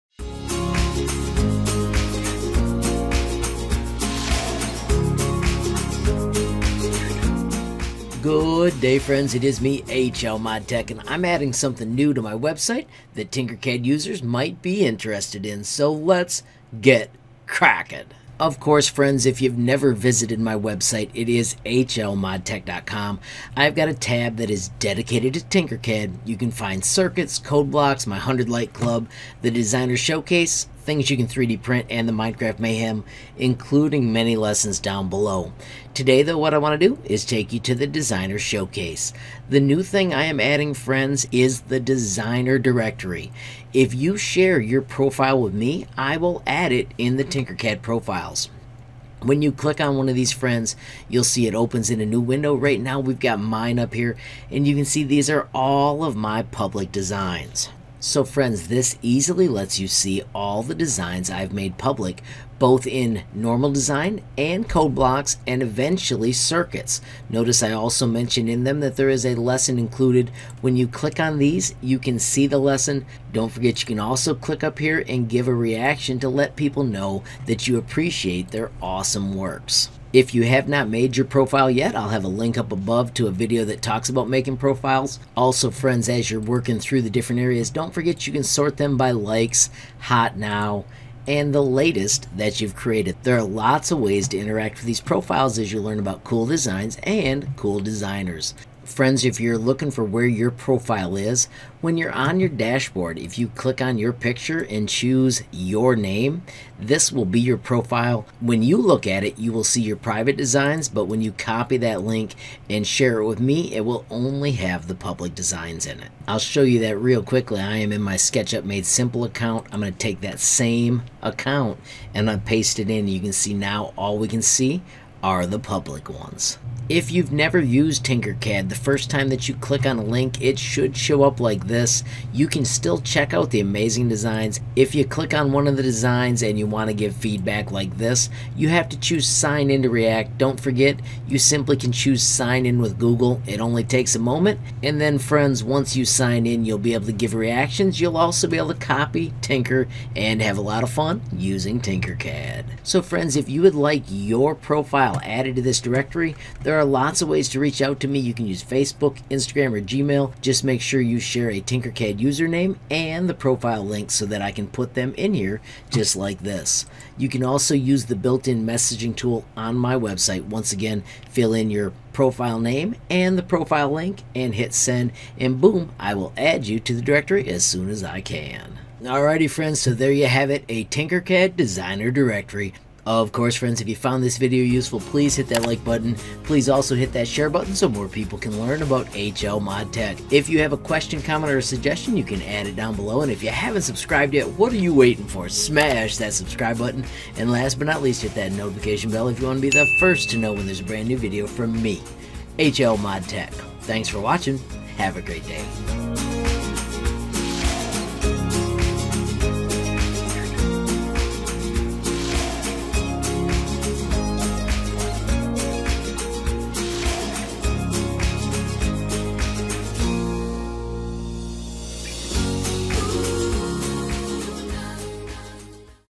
A Tinkercad Designer Directory | Share your Profile | Simple Steps
Have a glorious day and keep Tinkering! Tinkercad does not have A Tinkercad Designer Directory or a way to follow users. Today I created a Designer Directory so you can share your Profile and I will add it to my website.